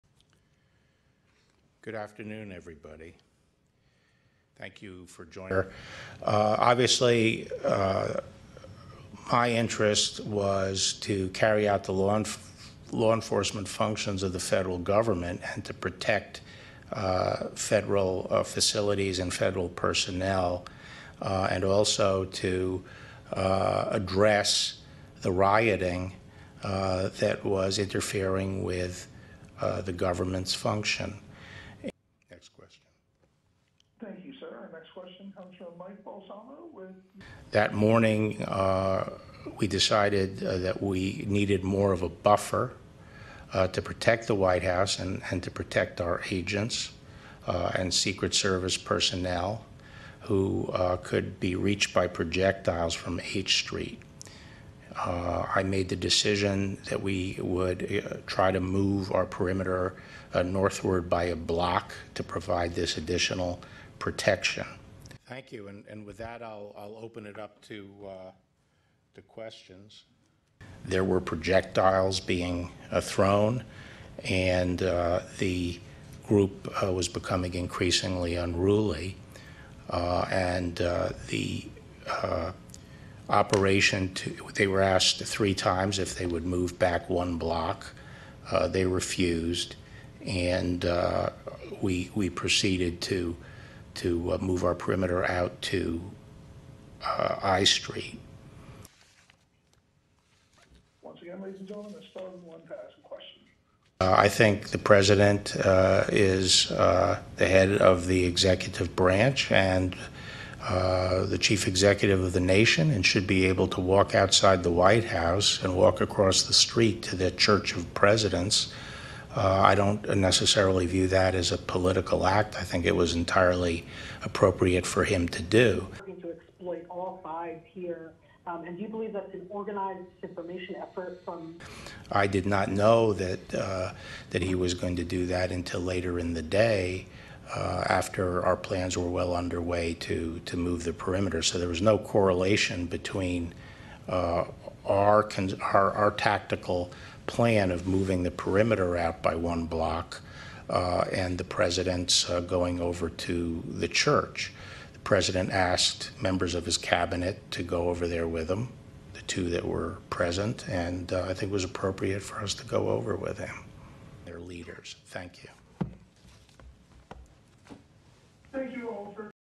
Barr defends violent perimeter move near WH
(4 Jun 2020) Attorney General William Barr defends the decision to move the perimeter near the White House on Monday that led to a violent clash with protesters.
"There were projectiles being thrown and the group was becoming increasingly unruly and the operation to which they were asked three times if they would move back one block," said Barr Thursday at the Justice Department.
"They refused. And we we proceeded to to move our perimeter out to I Street."
This comes after federal forces abruptly cleared peaceful protesters from Lafayette Park near the White House so that Trump could stage a photo op in front of St. John's, the “church of presidents,” holding up a Bible.
"I think the president is the head of the executive branch and the chief executive of the nation and should be able to walk outside the White House and walk across the street to the church of presidents," said Barr.
"I don't necessarily view that as a political act. I think it was entirely appropriate for him to do."
The protest come after George Floyd, who was black, died May 25 in Minneapolis after a white police officer pressed his knee into Floyd's neck while Floyd was handcuffed for several minutes even after Floyd stopped moving and pleading for air.
Since then, Floyd's name has been chanted by hundreds of thousands of people and empowered a movement.
In response, the Trump administration has made an effort to show a collection of force in Washington, manning the checkpoints and lining some streets with armored military vehicles.
The Justice Department had deployed agents from every one of its agencies, including the FBI’s Hostage Rescue Team, an elite tactical unit, and riot teams from the Bureau of Prisons.
The Park Police and Secret Service have had dozens of officers out in riot gear for the last few nights, in addition to the Metropolitan Police Department. U.S. Customs and Border Protection and other Homeland Security agencies were also dispatched.
Most of the protesters have been peaceful and tried to discourage violence.
Trump, Barr and others have tried to blame some of the civil unrest on left-wing extremist groups, including antifa, and other “anarchists.” Short for anti-fascists, antifa is an umbrella term for far-left-leaning militant groups that resist neo-Nazis and white supremacists at demonstrations.